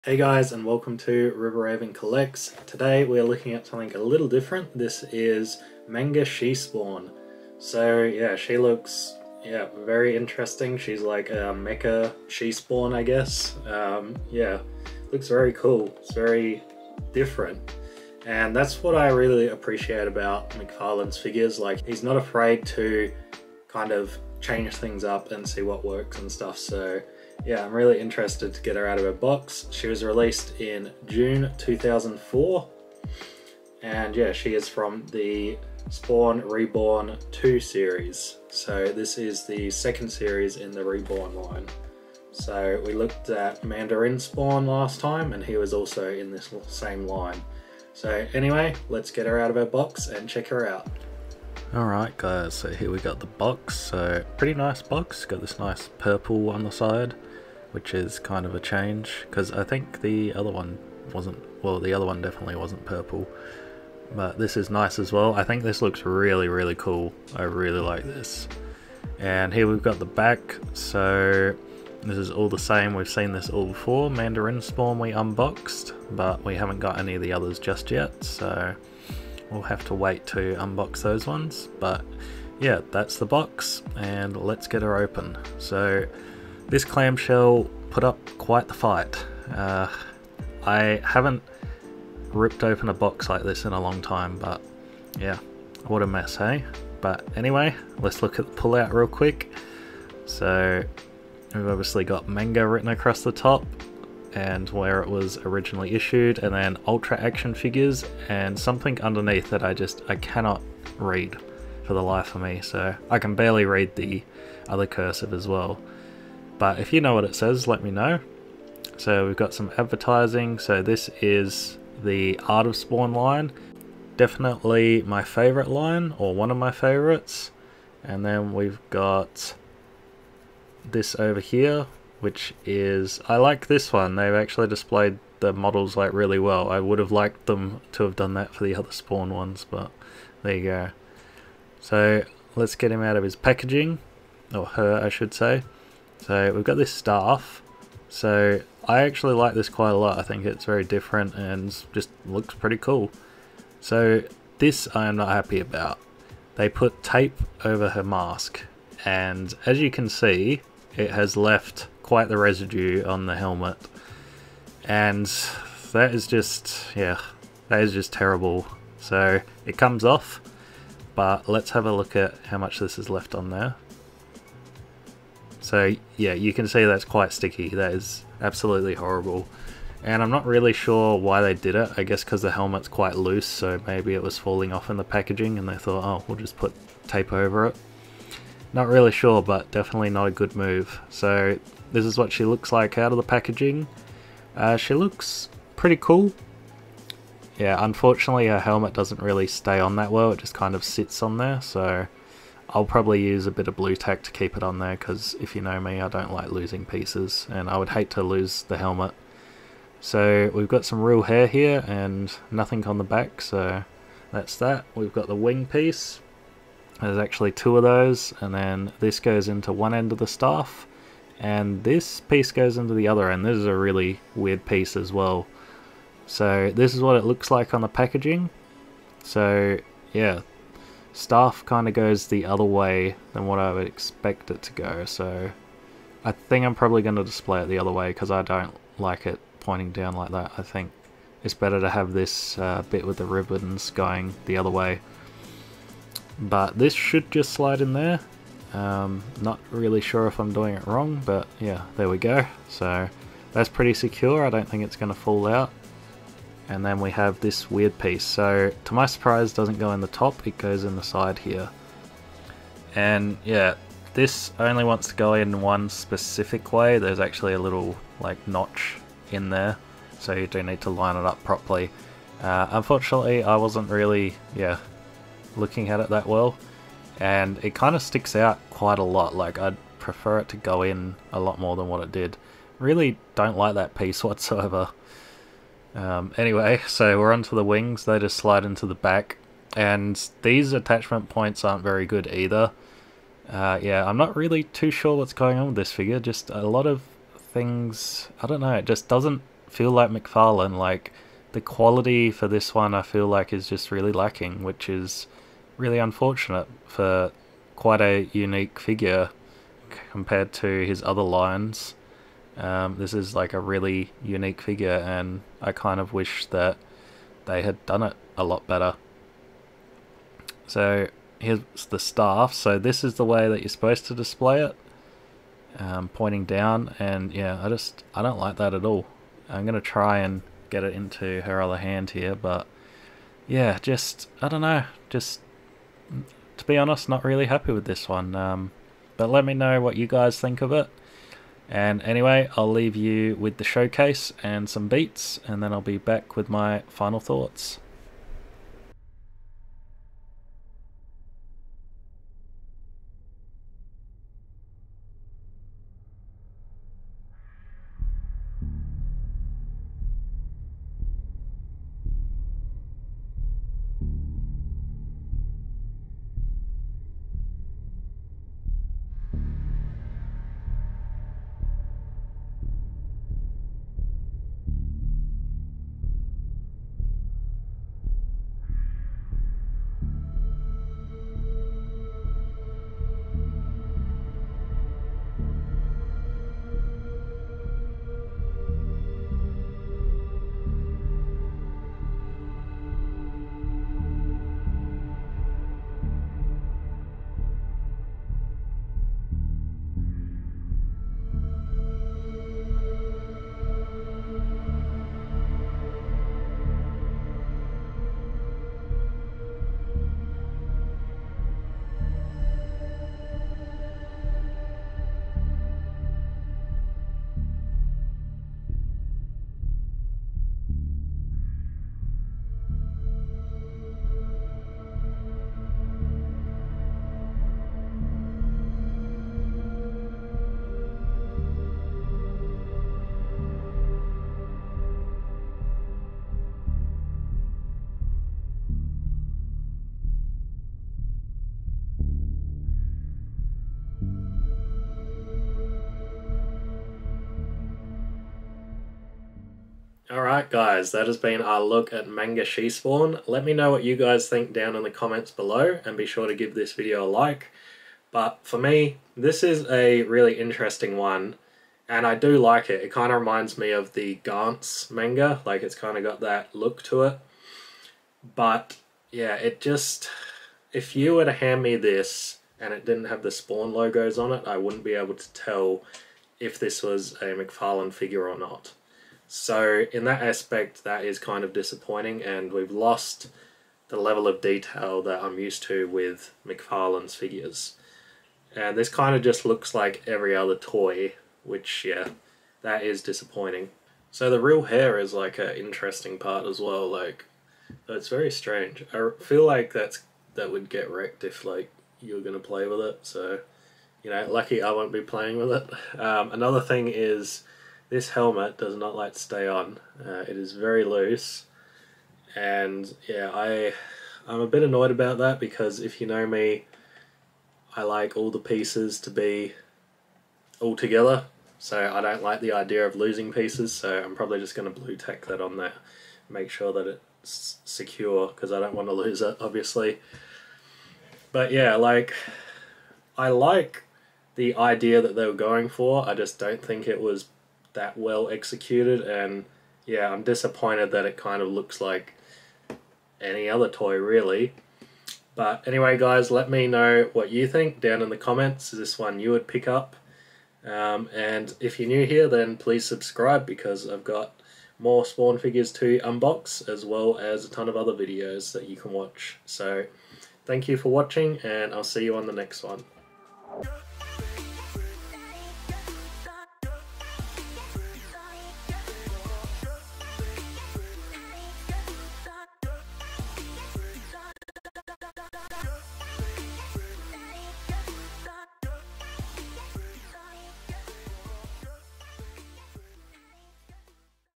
Is this still Mcfarlane?!? Manga She-Spawn | Spawn Reborn Series | Release June 2004
Hey Guys
I've got alot more to unbox so seeya on the next one!

Camera: Sony ZV - 1
Editing software: Filmora
mic: Razor Siren

Spawn | She-Spawn | Manga | Mcfarlane toys | retro figure